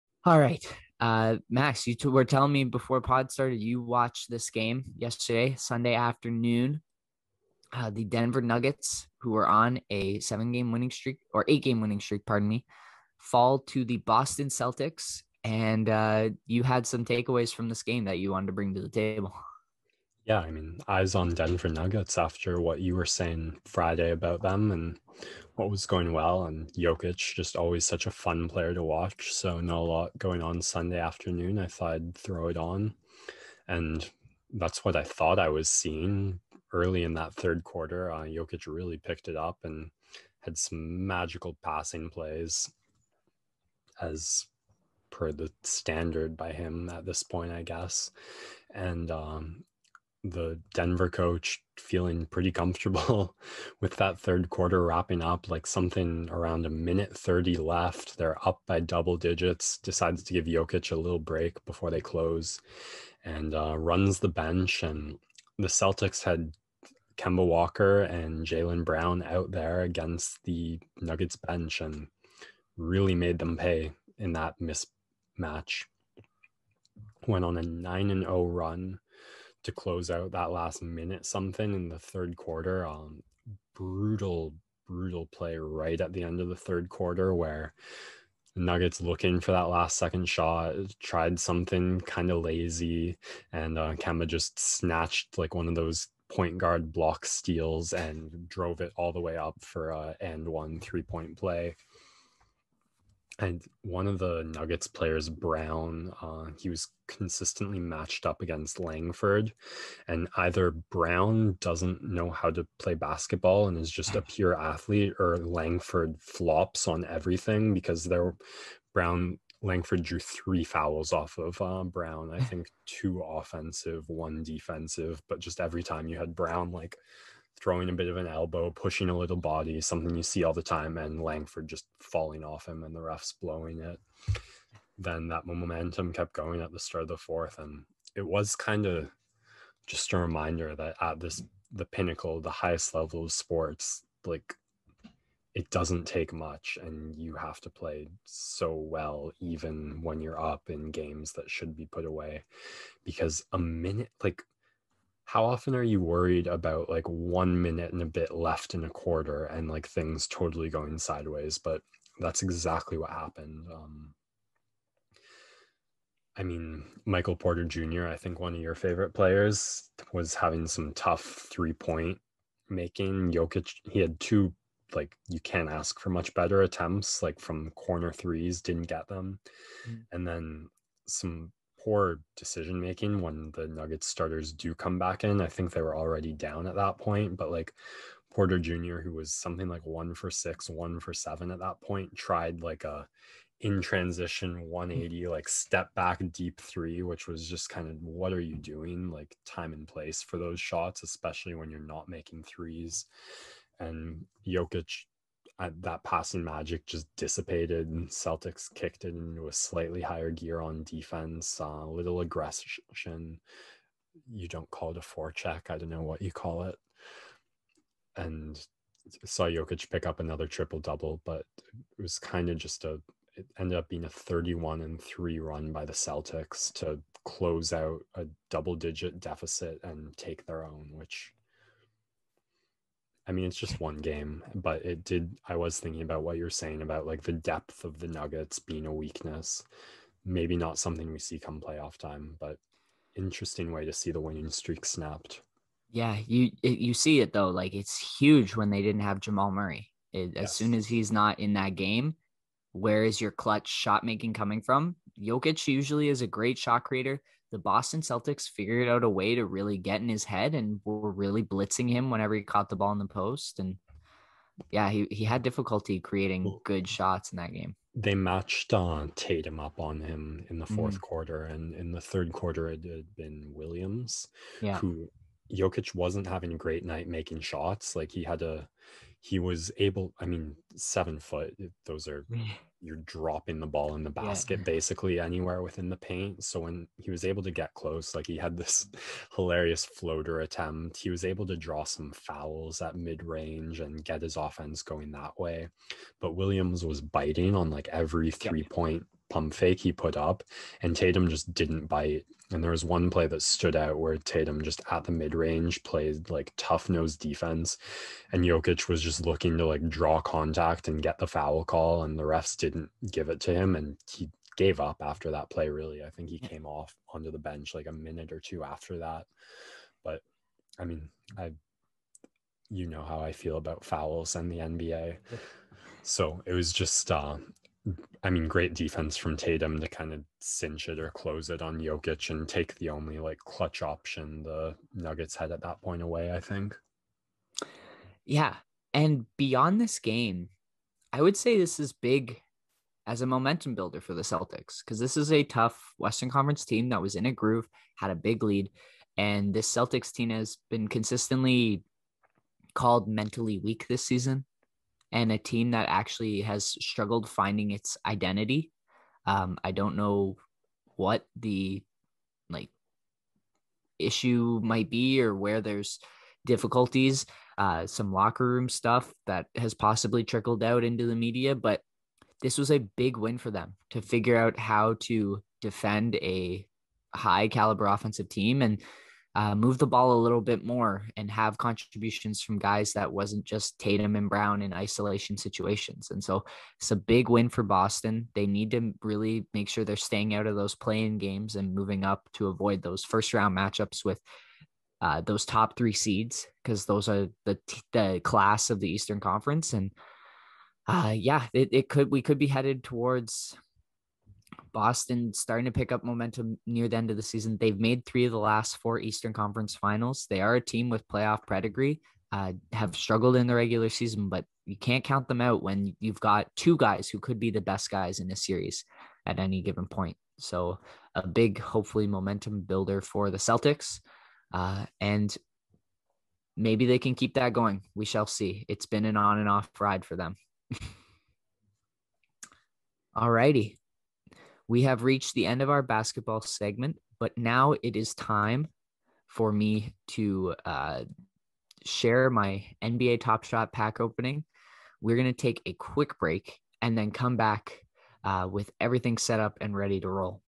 Celtics 31-3 Run Ends Nuggets Win Streak
We Recap Sundays Boston Celtics Denver Nuggets game which saw a 31-3 run by the Celtics put the game away.

Clip Taken from the Sports NextDoor Podcast.